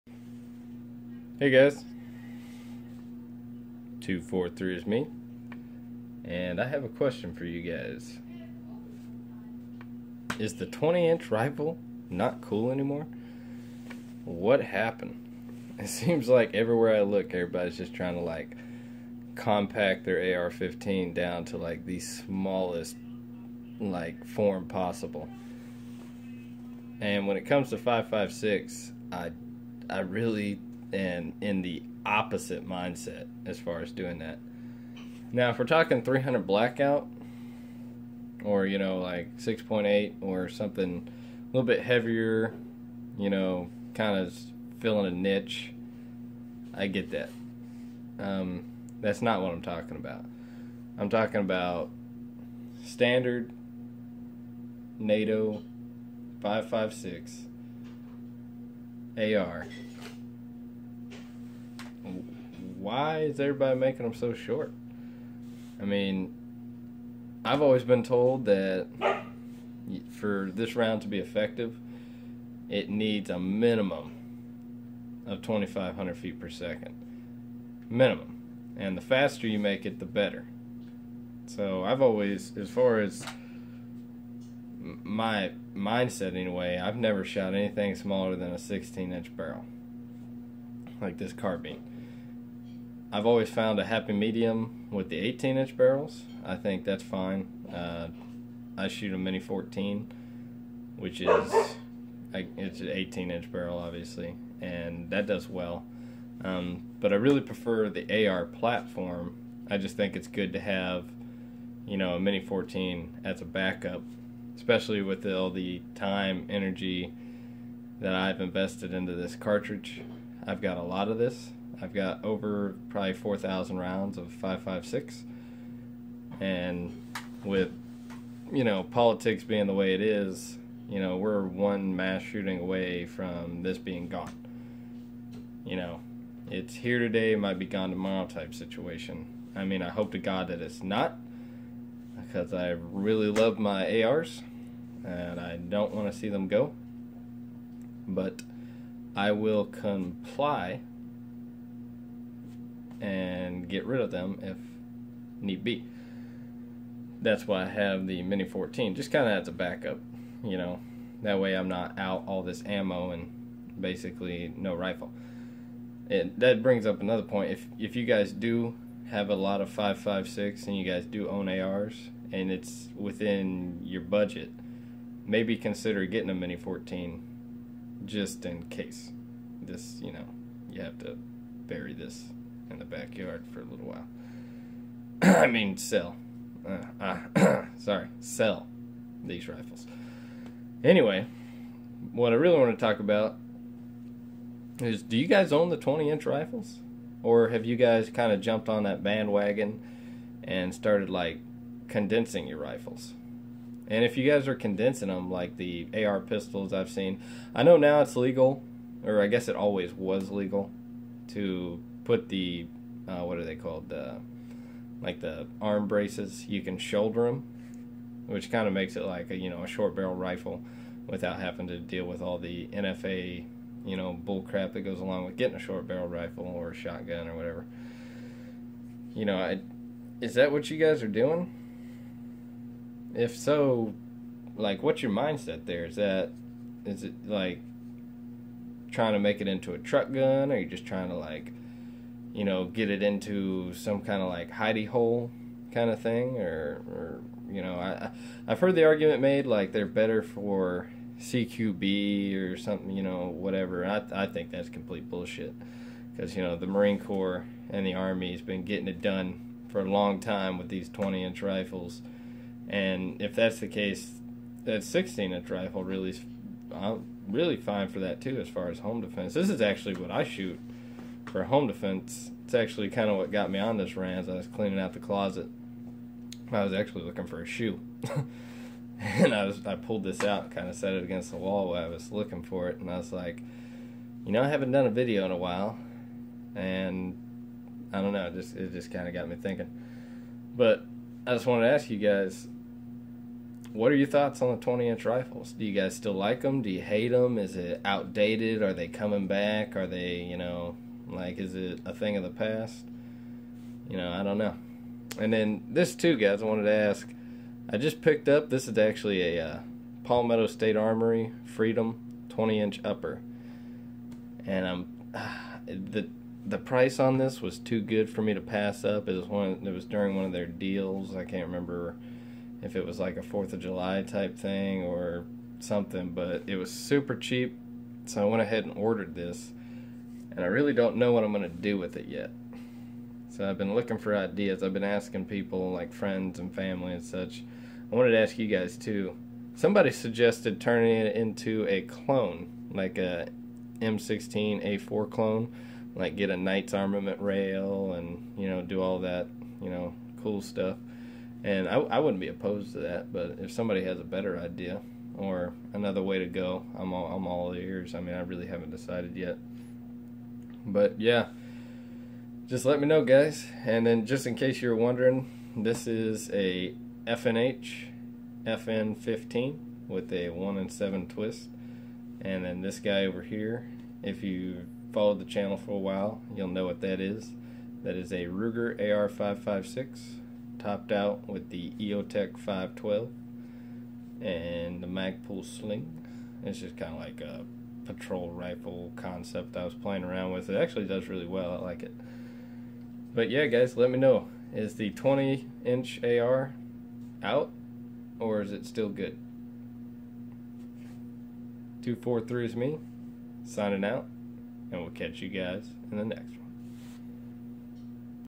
Is the 20in AR not cool anymore?
I'm asking your guyses opinion about 20 in AR rifles what do you think yay or nay?